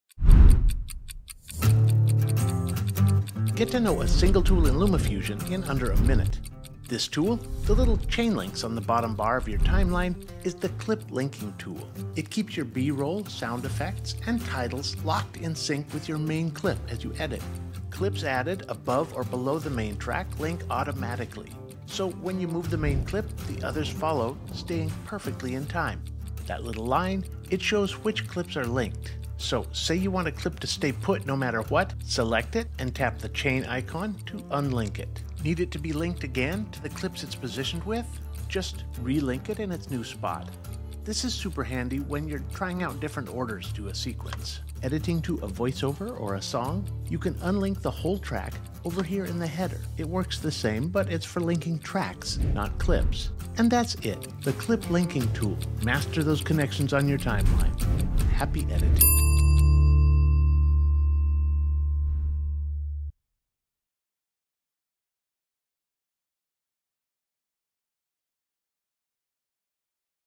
Tool a Minute: The Clip Linking Tool—Secret Handshake for Mastering the Enhanced Magnetic Timeline.
Ever get confused about why clips sometimes bring other clips along when you move them?

Welcome to "Tool-a-Minute", where we take a QUICK look at a single tool in LumaFusion's user interface, show you where to find it, what it does, and when you might want to use it.

In this episode: Learn about Clip Linking in LumaFusion, and when and how to use it to streamline your editing. Find out more in the next minute of your life...

Here's a bit deeper dive to help you make the most of this super useful tool

And as always, if you have any questions, please get ahold of us via our Support channel (tap 'email Support'). We want to hear from you!